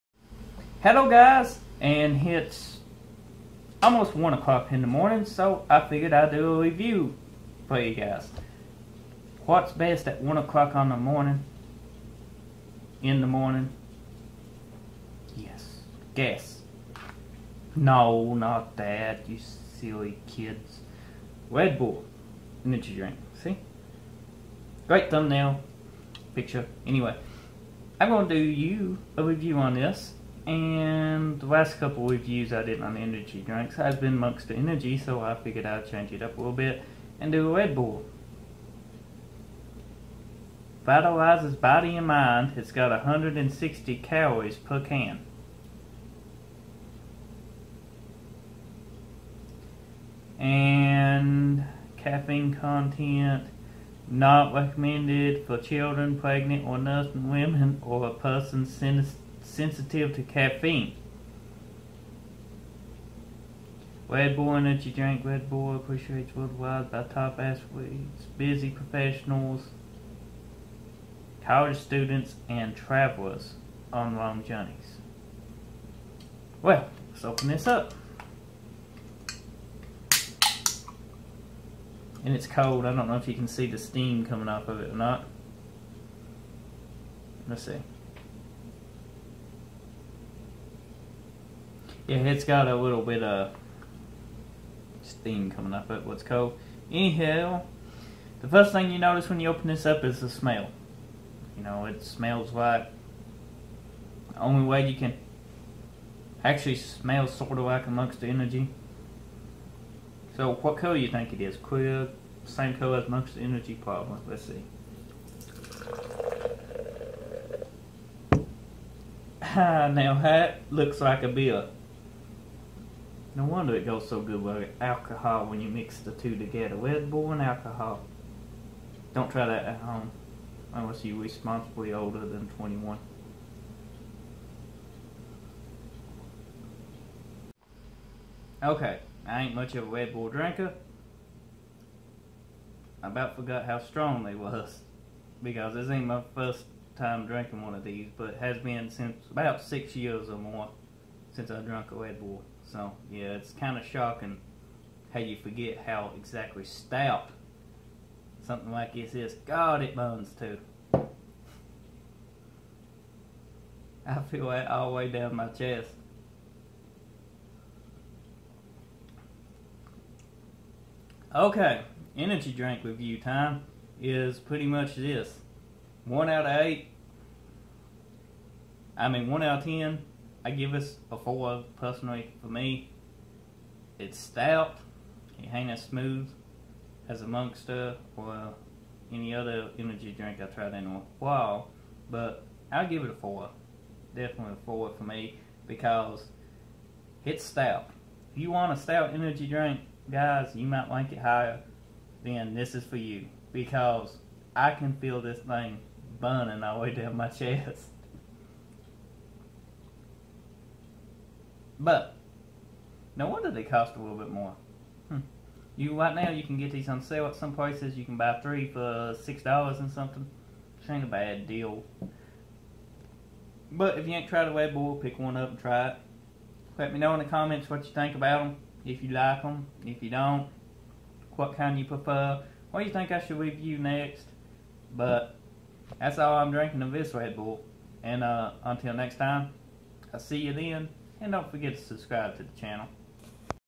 Red Bull Energy Drink Food Review GIVES YOU WINGS
Today we review the red bull energy drink, Gives you wings